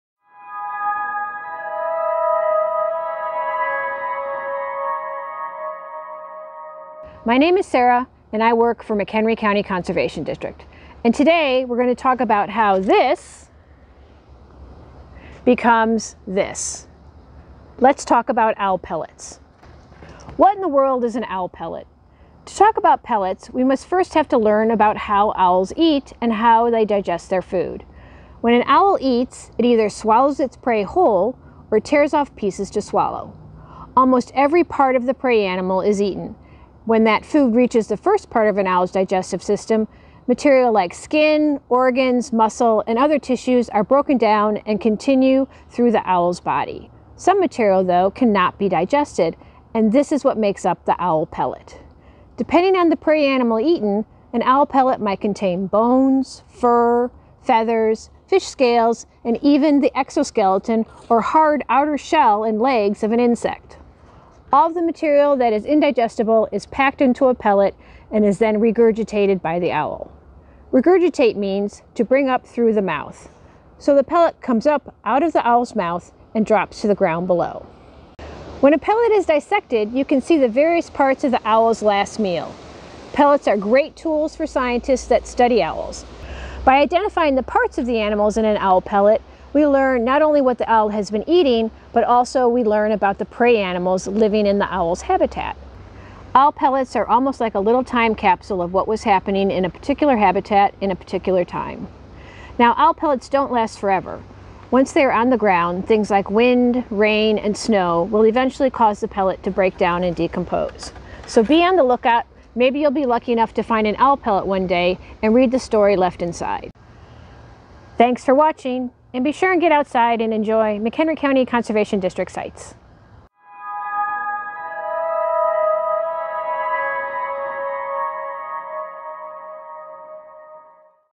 All About Owls: Pellets
Join our Wildlife Resource Center staff as they take a look at the owls of McHenry County including several specific species and various adaptations such as their eyesight, hearing, and feathers. For more information about our Wildlife Resource Center visit MCCD.me/Wildlife.